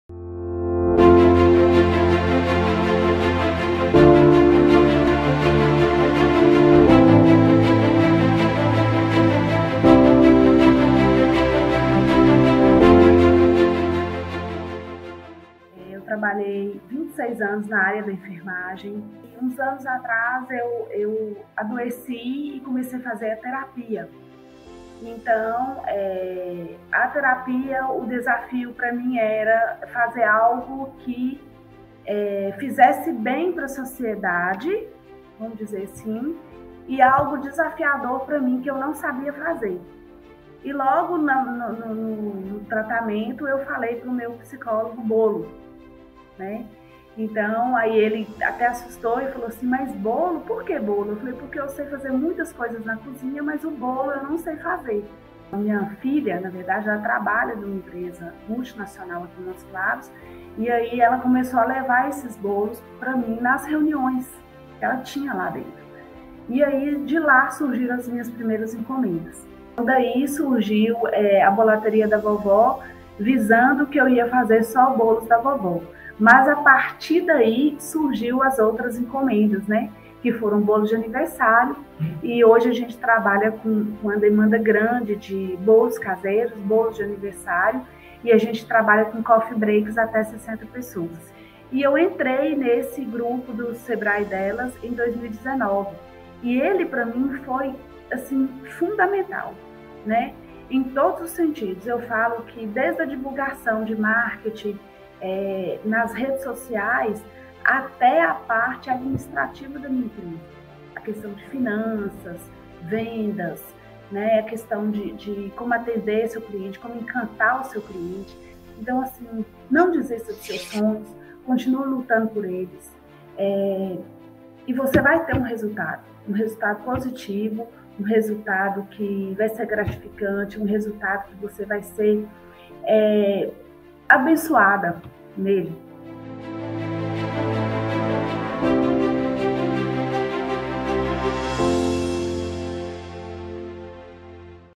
Conheça a história de Lilianne Silveira, da Bolateria da Vovó
Em 2017, Lilianne Silveira começou a fazer bolos caseiros como forma de terapia e de presentear família e amigos. As primeiras encomendas vieram rápido: a filha da empreendedora levou um dos bolos da mãe para uma reunião de trabalho, e os colegas da empresa pediram mais!

Foi assim que a atividade caseira de uma técnica de enfermagem se transformou na confeitaria Bolateria da Vovó (@bolateria_da_vovo), de Montes Claros (MG). Hoje, Lilianne atende um grande número de encomendas de bolos, doces, cestas de presente, coffee break e kits festa. E o afeto ainda está presente em cada produto.

Lilianne conta que enfrentou uma série de desafios no caminho, como a captação de clientes. A solução foi investir em capacitação, para melhorar a prospecção, o atendimento e a fidelização. Nesse movimento, ela participou da turma do Sebrae Delas.

Participe também do Sebrae Delas. Procure o Sebrae mais próximo de você, e vamos juntas!

Assista ao vídeo, curta e compartilhe! Siga nosso canal e ative as notificações para ficar por dentro de novos vídeos! FiqueLigado!

Acesse e conheça todas as nossas soluções!